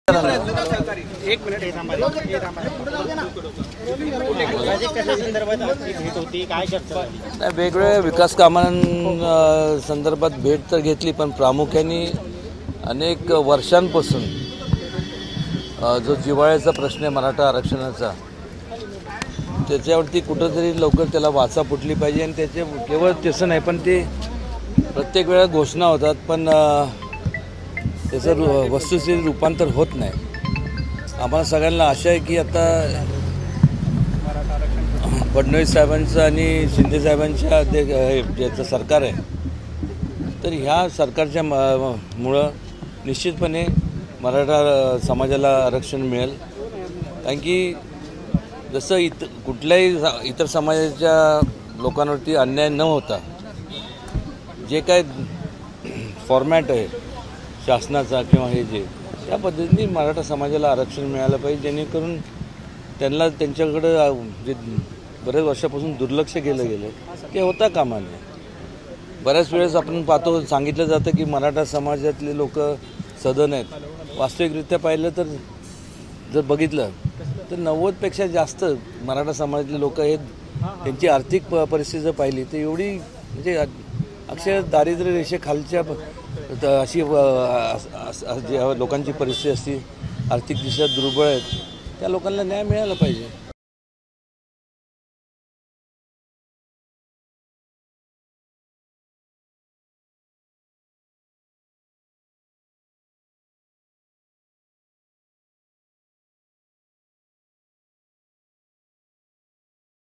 Udayan Raje Bhosale : मराठा समाजाची स्थिती दयनीय | LetsUpp Marathi
मराठा समाजाची स्थिती दयनीय
मराठा आरक्षणाच्या फक्त घोषणा…पहा नेमकं काय म्हणाले उदयन राजे भोसले